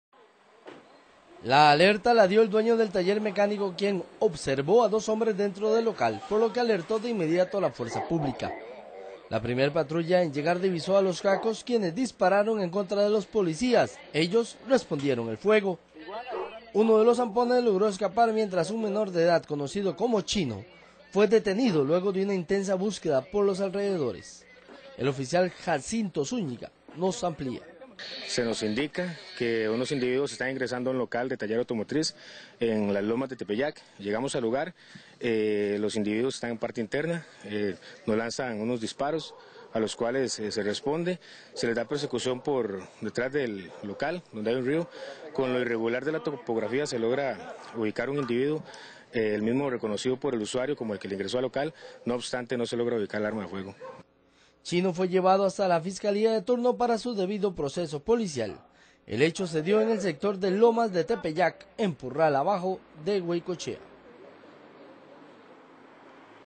ROBO TALLER
Un menor de 17 años trato de robar en un taller Mecánico en el sector de Goicoechea pero la fuerza pública frustro el  hecho y detuvo al caco.